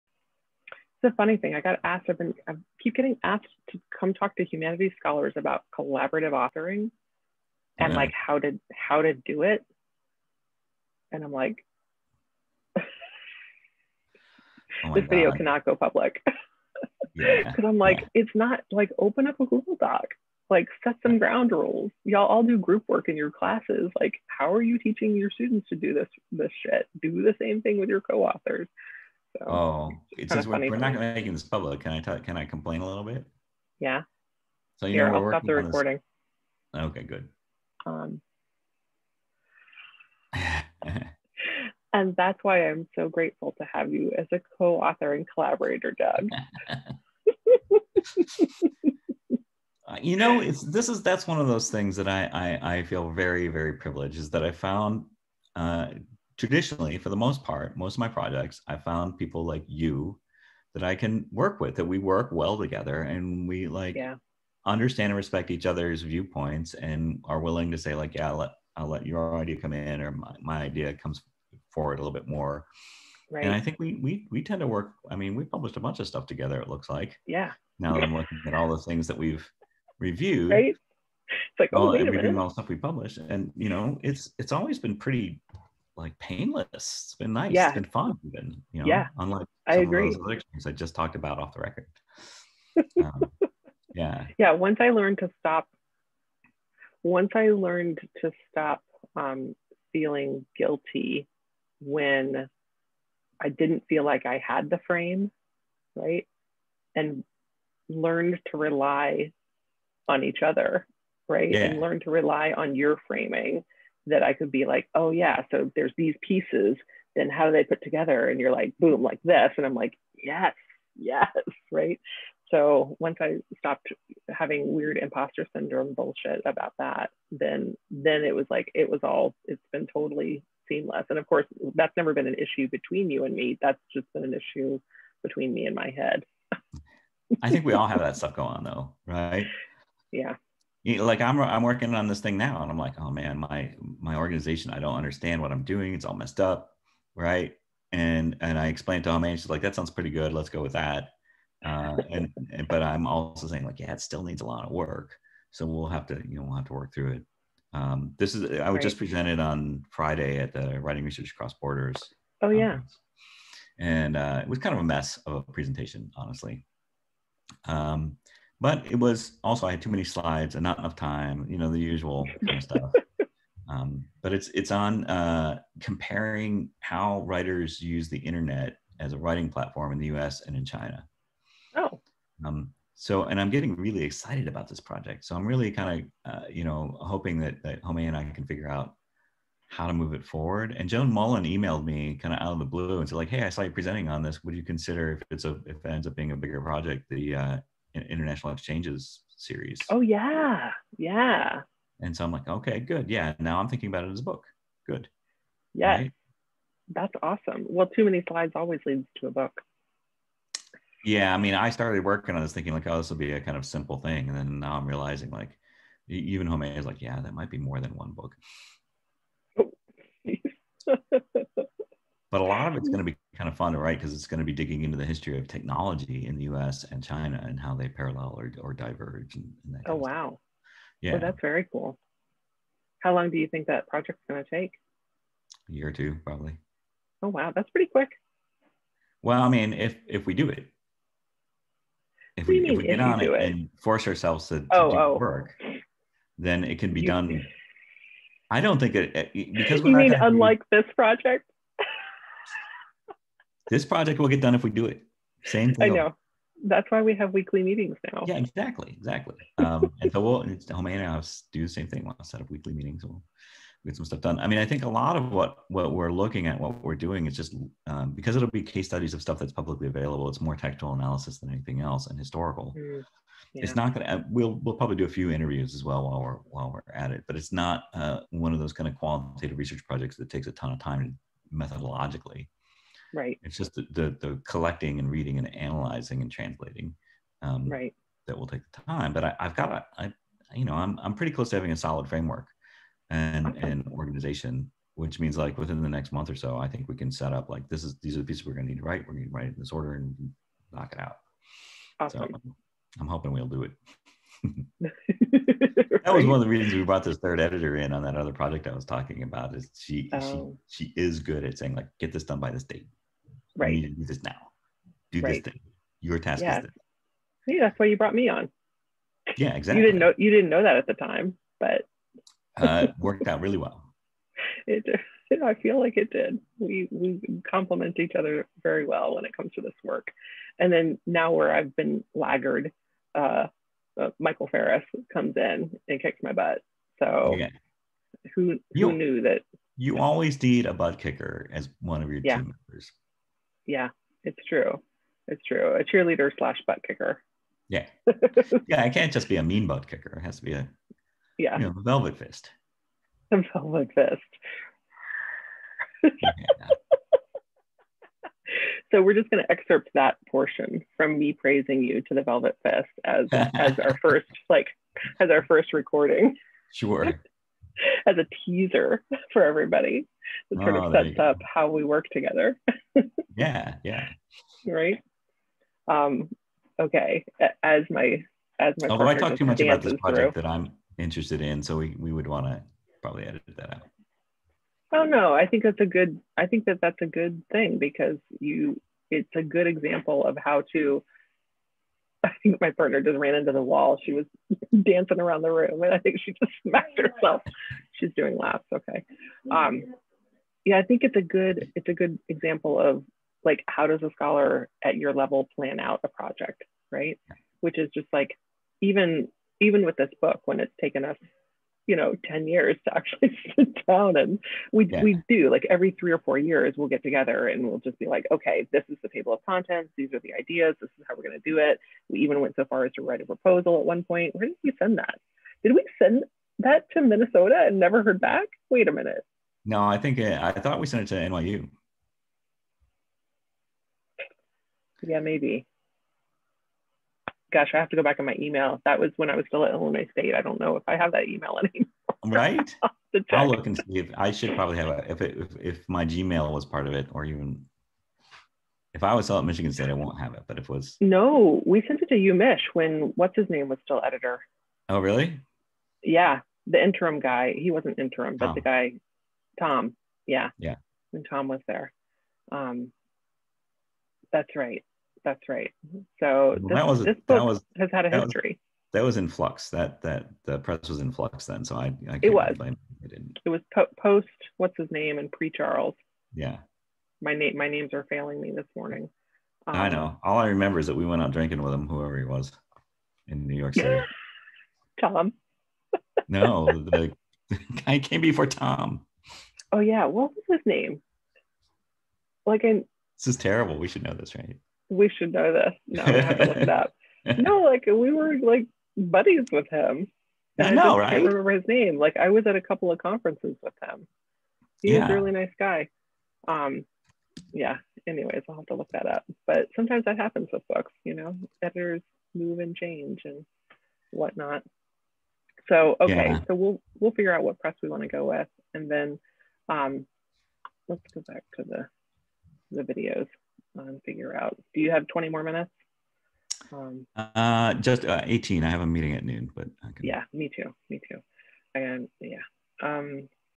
Co-Authoring and Collaboration with the Kairos Editors
Kairos Senior Editor Doug Eyman and Editor Cheryl Ball talk about co-authoring, collaboration, devising a book idea, and how long it takes to get a book to press (if they can even remember what press they sent it to). This video is a roughly edited test episode of the oral histories the Kairos Editors are conducting as they begin to write the history of Kairos and digital publishing more broadly. (Closed captions to come on this video -- we recorded it incorrectly so it didn't auto-transcribe.)